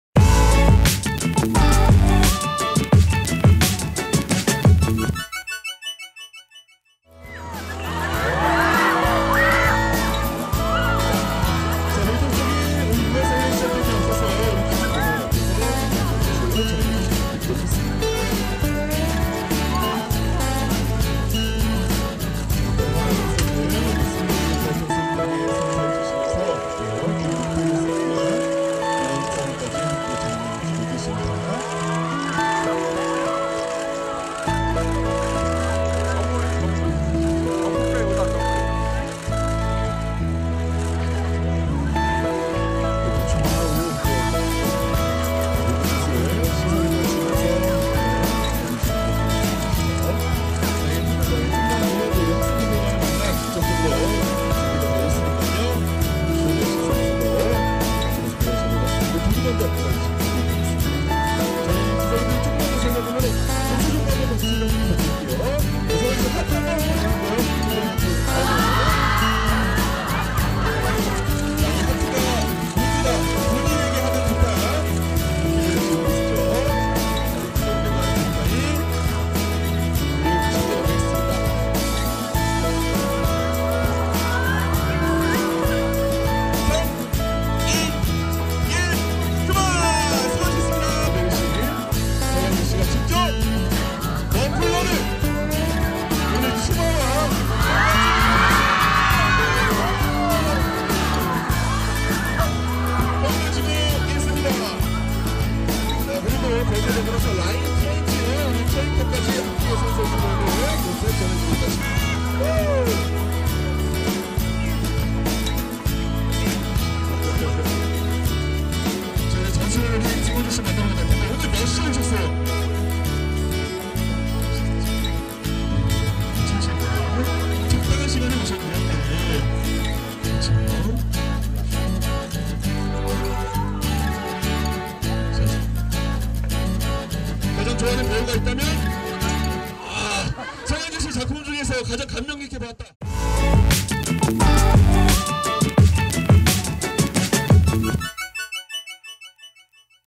[HD영상] 서강준(Seo Kang-joon) 팬사인회
12일 오전 숙명여자대학교에서 열린 ‘서강준이 토익책 주는 응원 사인회’에 참석해 팬사인회에 참석했다.

TOPSTARNEWS.NET